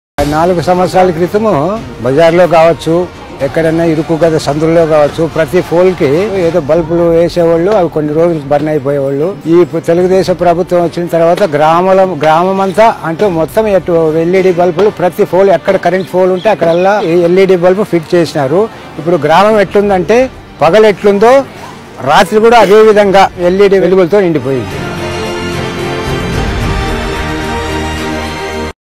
LEDs at Villages in Andhra Pradesh
Villages across Andhra Pradesh have been brightly lit with LEDs leading to decrease in crime rate and accidents